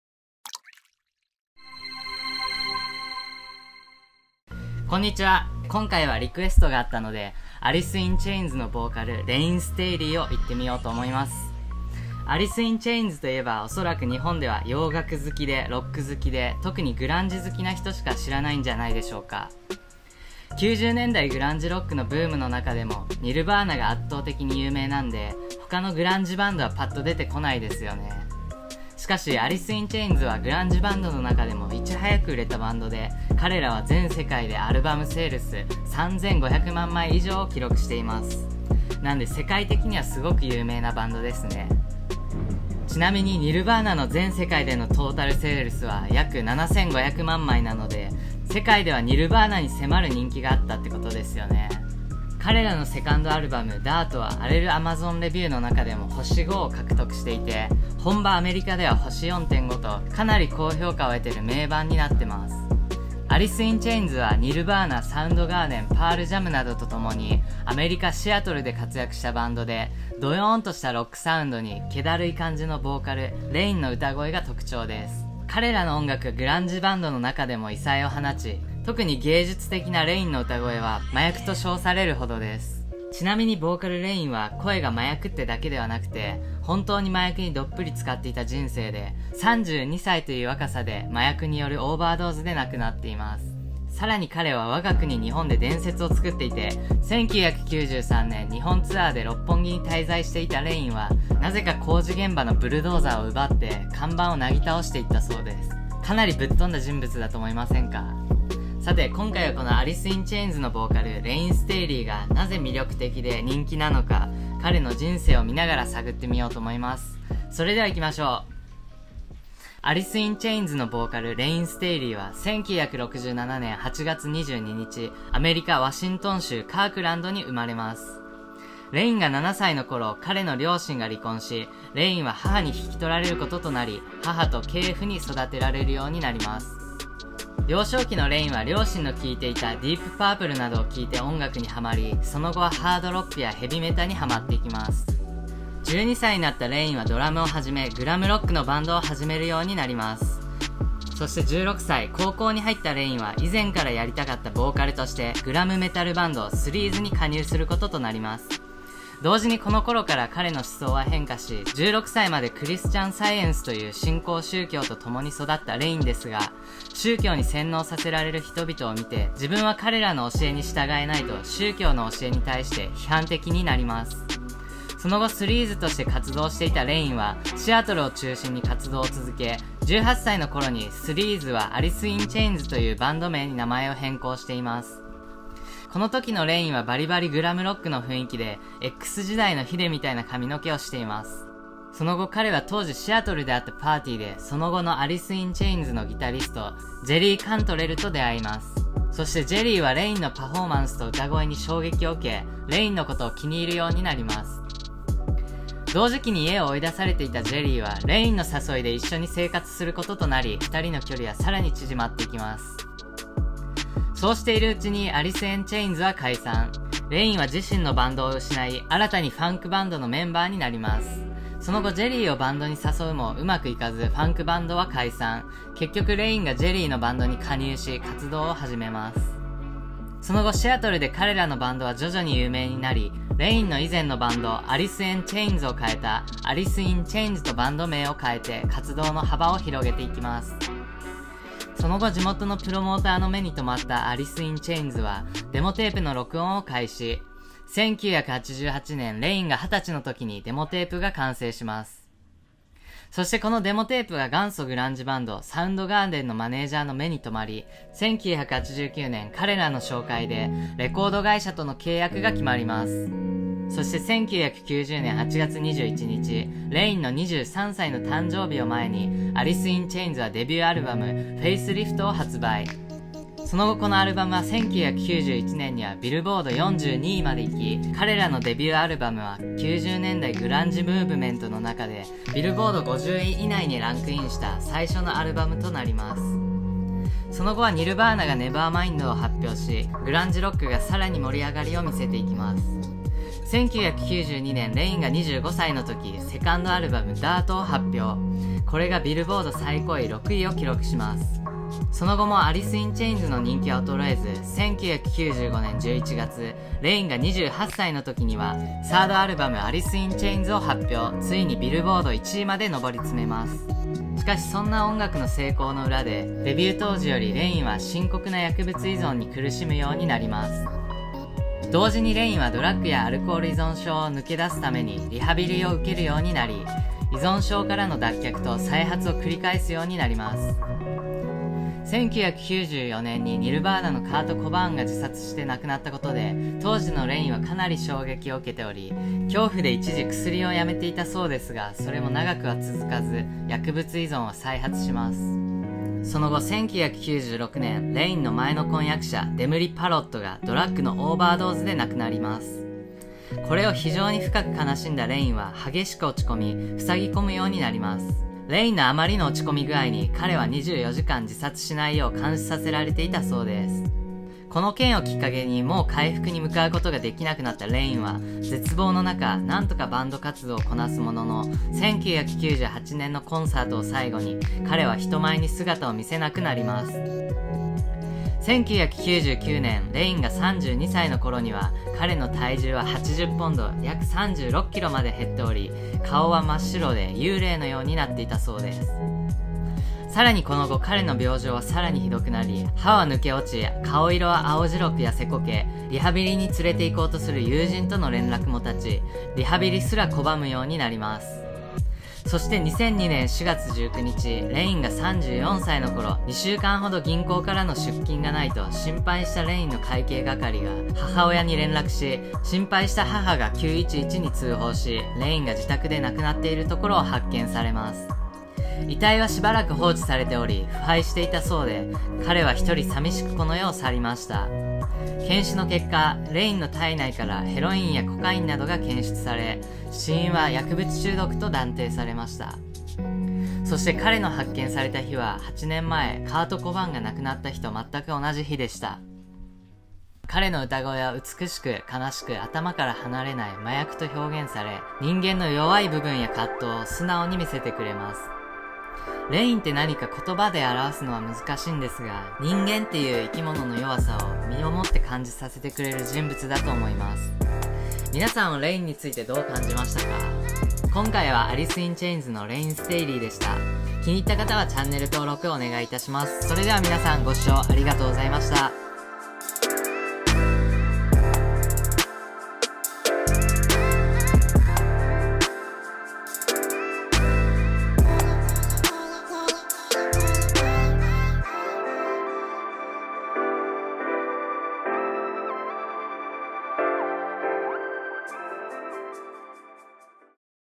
Alice in Chains アリス・イン・チェインズ レイン・ステイリーの生涯 〜人物伝〜
90年代グランジ・ブームに活躍したAlice in Chainsのボーカル、レイン・ステイリーの波乱万丈の生涯を解説！！